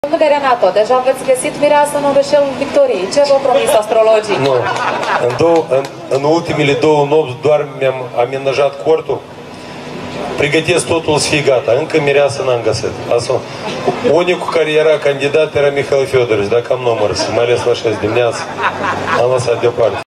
Igor Dodon și Renato Usatîi răspund în direct la întrebările cetățenilor din Orășelul Moldovei - ...
Se caută mireasă pentru primarul mun. Bălți :D
Titlul evenimentului: Igor Dodon și Renato Usatîi răspund în direct la întrebările cetățenilor din Orășelul Moldovei - Victoriei